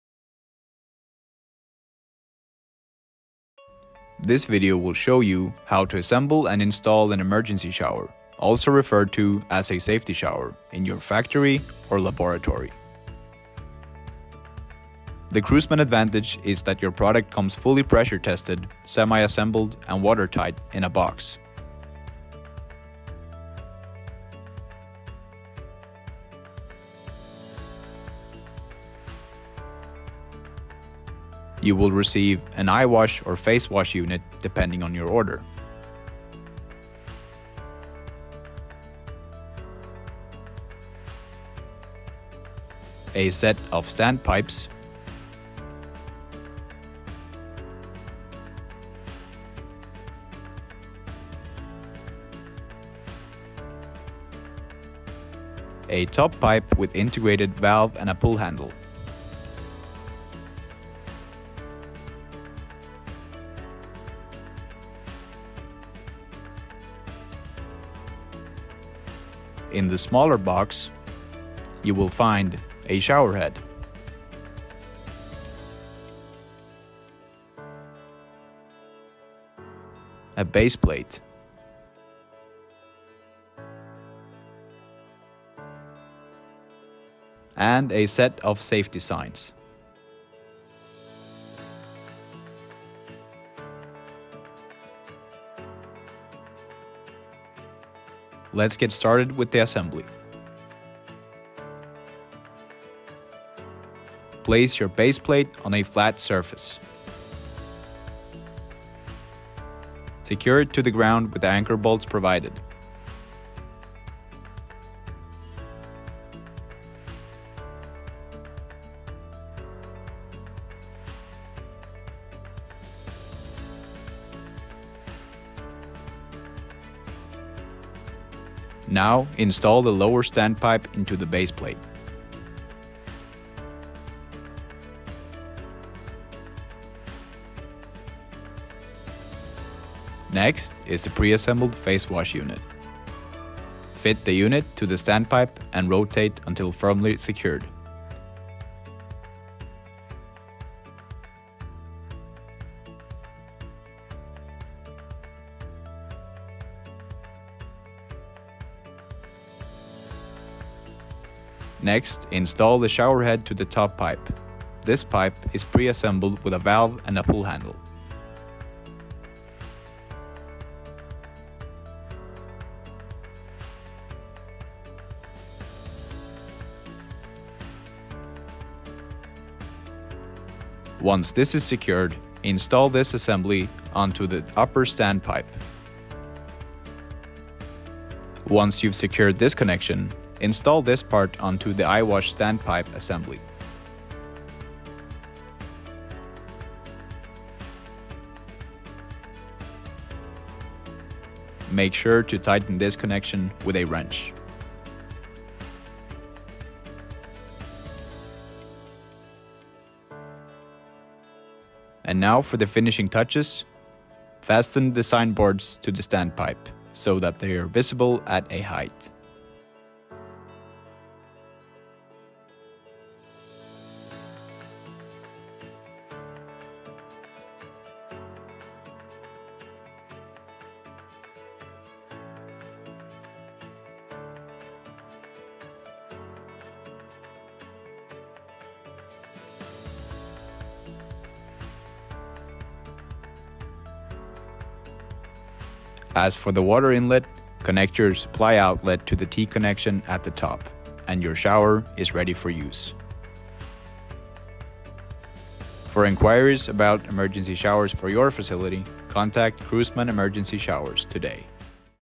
How to install an Emergency shower / Safety shower | Emergency shower installation | Krusman
This video will show you how to assemble and install an emergency shower, also referred to as a safety shower, in your factory or laboratory. Krusman emergency shower installation video timestamps:

1. Introduction                                  0:00
2. Components                                 0:13
3. Installation                                    1:38
4. External water connection         4:05
5. Contact                                         4:15